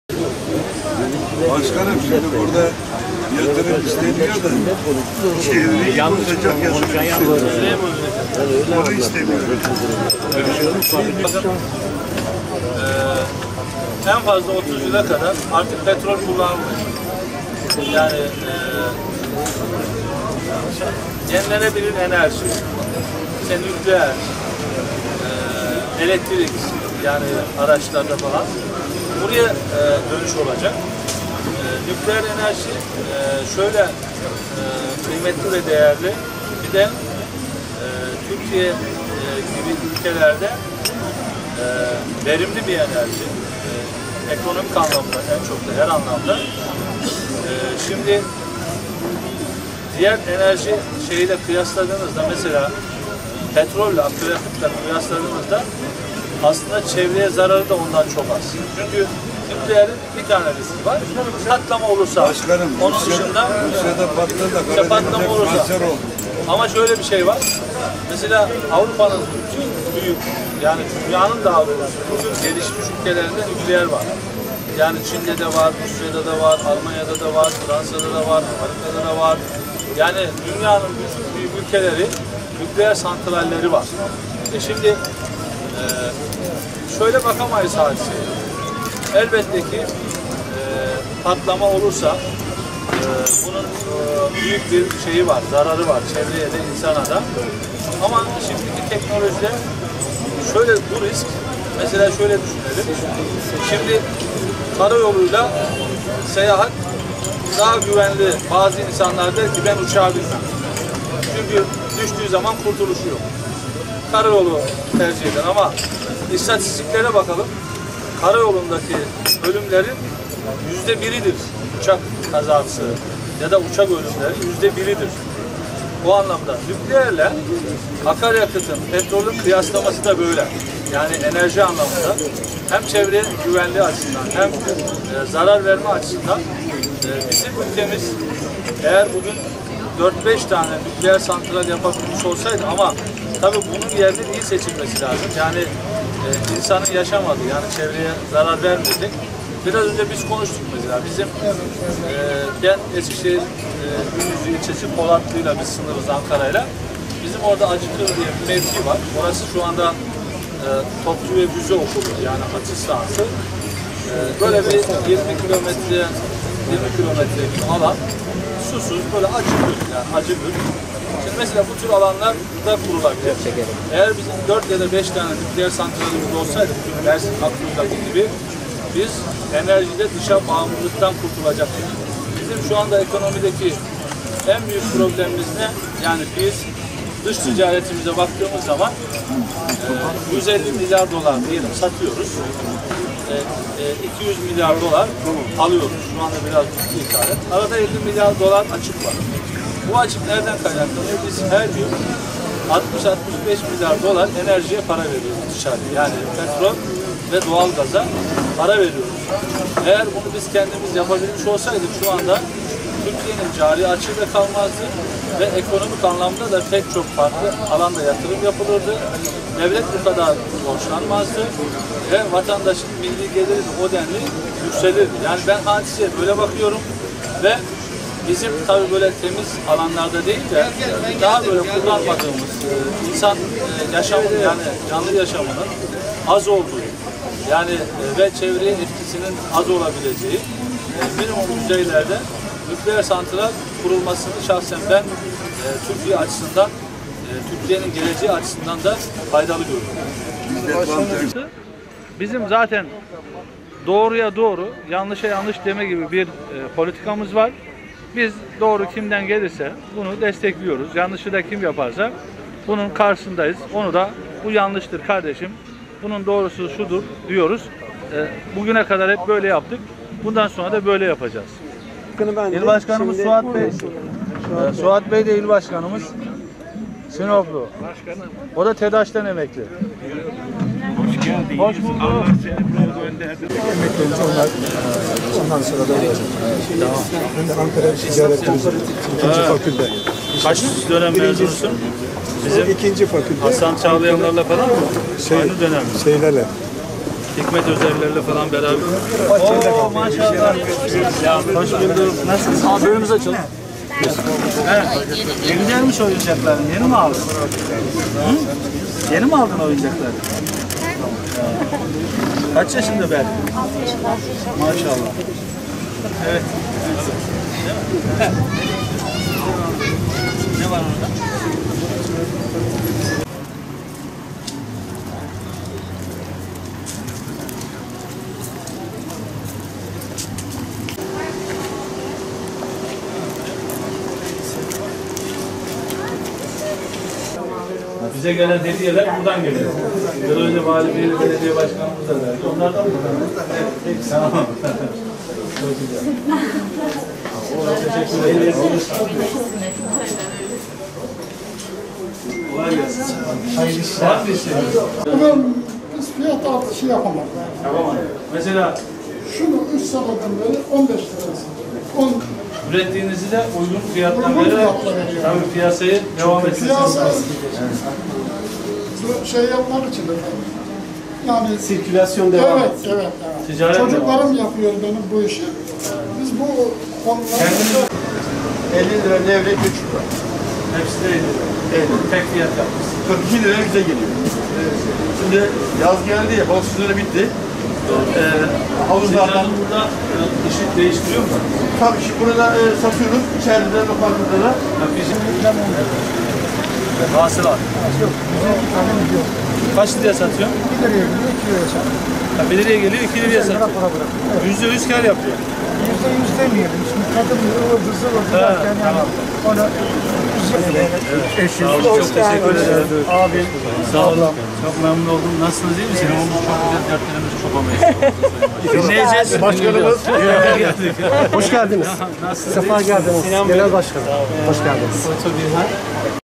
Destici'den Sinop'ta nükleer açıklama
Sinop'ta konuşan BBP Genel Başkanı Mustafa Destici; “Eğer bizim 4 ya da 5 tane nükleer santralimiz olsaydı Mersin Akkuyu’daki gibi biz enerjide dışa bağımlılıktan kurtulacaktık. Nükleere karşı değilim ancak, insandan daha uzak alanlarda bu yatırımların yapılması taraftarıyım”